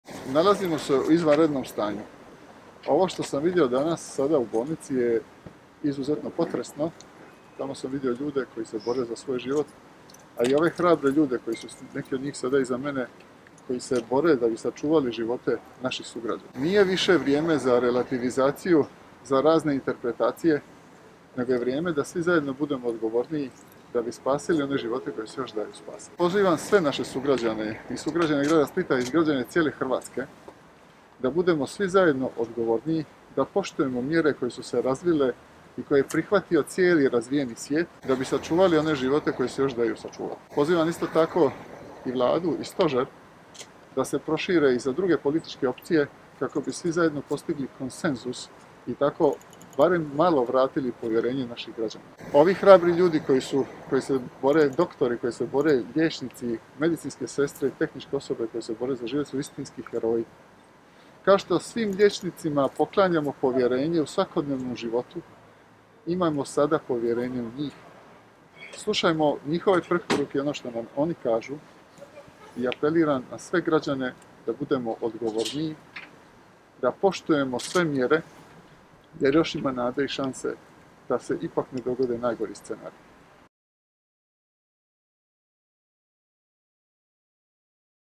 Ivica Puljak: Nalazimo se u izvanrednom stanju
Gradonačelnik Splita Ivica Puljak, ravnatelj KBC-a Split Julije Meštrović i  predstojnik Klinike za infektologiju KBC Split Ivo Ivić na izvanrednoj konferenciji za medije ispred Klinike za infektologiju na Križinama.
Glavna tema je aktualna situacija s virusom COVID-19.